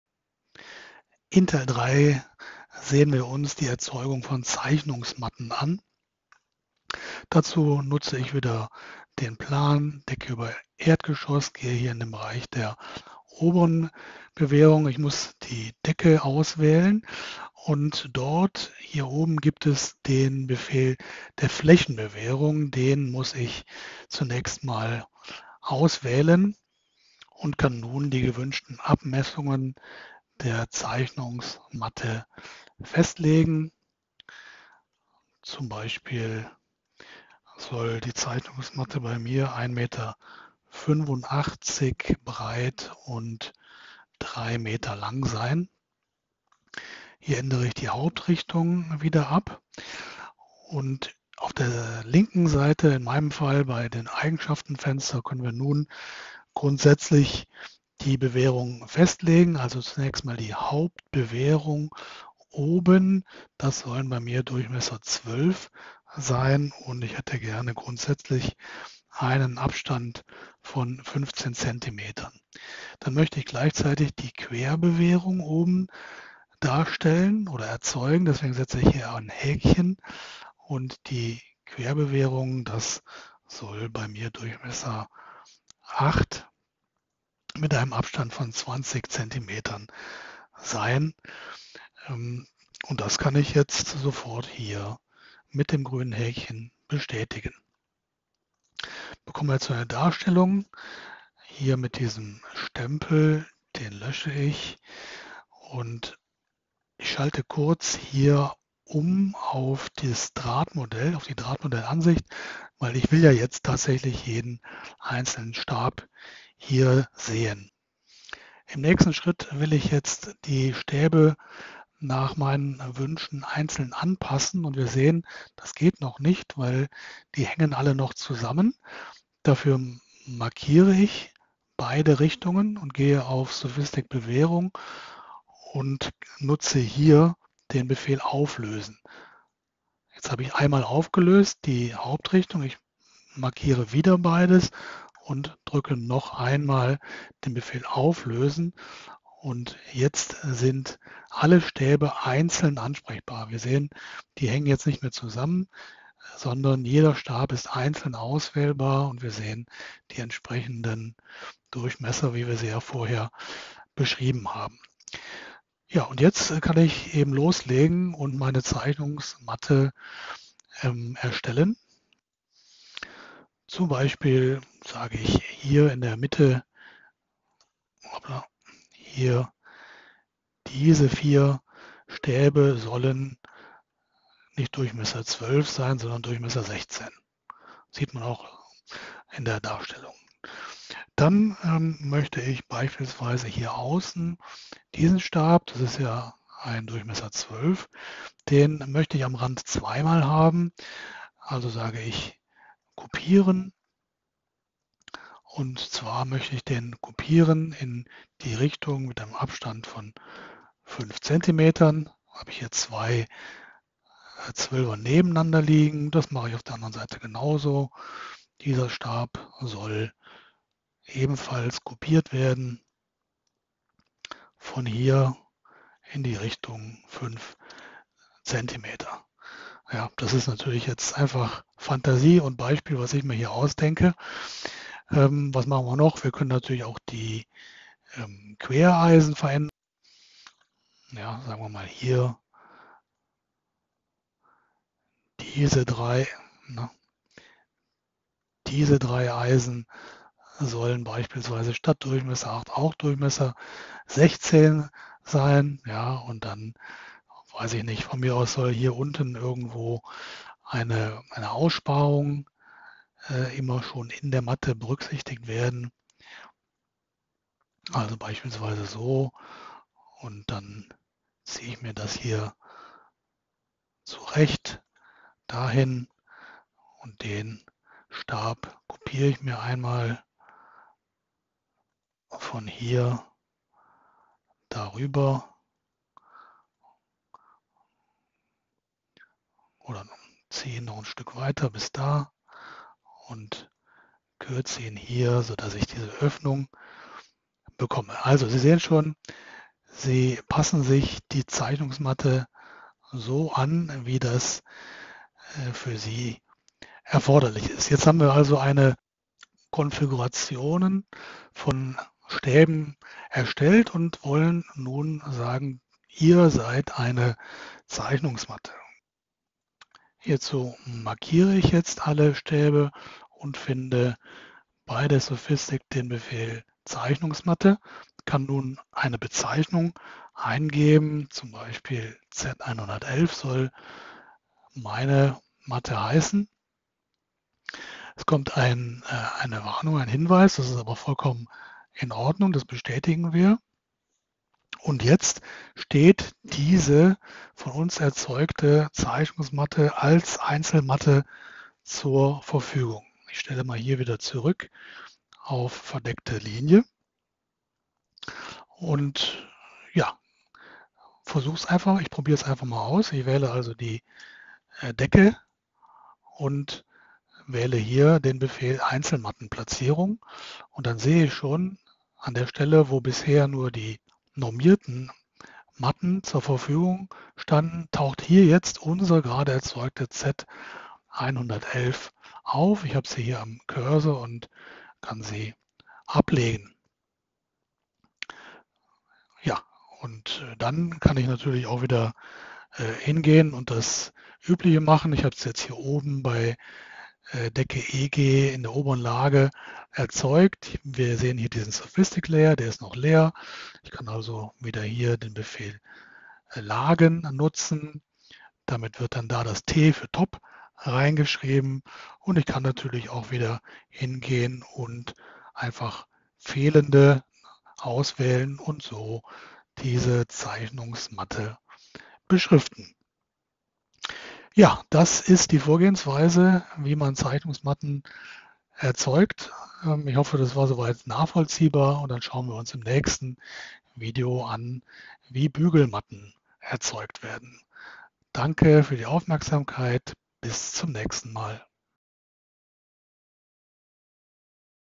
Bewehren Teil 3 - Zeichnungsmatten mit Revit und SOFiSTiK
In diesem SOFiSTiK Video Tutorial sehen wir uns die Erzeugung von Zeichnungsmatten an. Der Weg führt zunächst über die Revit Funktion "Flächenbewehrung". Als Erstes werden die gewünschten Abmessungen der Zeichnungsmatte sowie eine Ausgangsbewehrung in Haupt- und Querrichtung festgelegt. Für den nächsten Schritt ist die SOFiSTiK Funktion "Auflösen" erforderlich. Dadurch werden alle Einzelstäbe separat ansprechbar. Nun kann die Zeichnungsmatte nach den eigenen Wünschen durch Kopieren, Verschieben und Löschen der Einzelstäbe und Ändern der Stabdurchmesser frei konfiguriert werden. Schließlich wird die so erstellte Stabkonfiguration über den zugehörigen SOFiSTiK Befehl in eine Zeichnungsmatte mit wählbarer Bezeichnung umgewandelt. Ab sofort steht die Zeichnungsmatte nun als Einzelmatte zur Verfügung und kann genau wie normierte Matten verwendet werden.